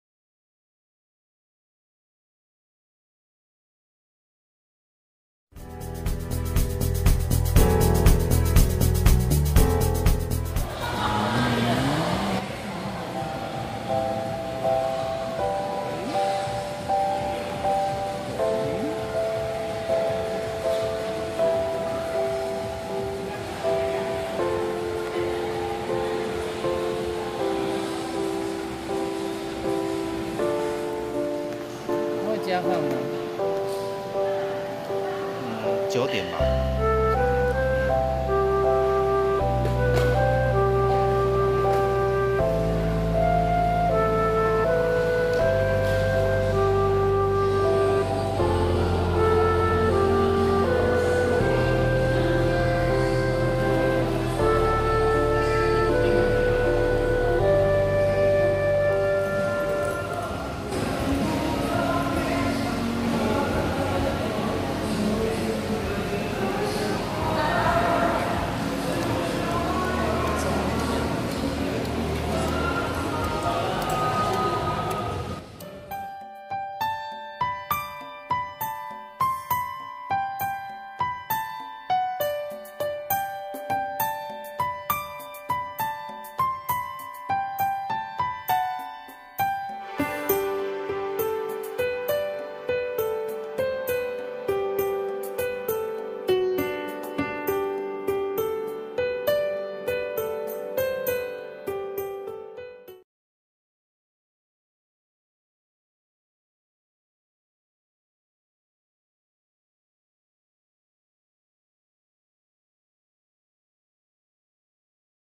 釣泰國蝦"吃蝦"處理方式千萬注意..Eat Thai shrimp to pay attention
大家都很喜歡釣泰國蝦 但你知道吃牠的時候要注意甚麼嗎
這次影片就帶大家了解一下，蝦頭有一個黑囊千萬不要吃
因為所有蝦的雜質都會累積在這邊，我們也提供了方式
下次大家吃的時候就特別注意一下喔！！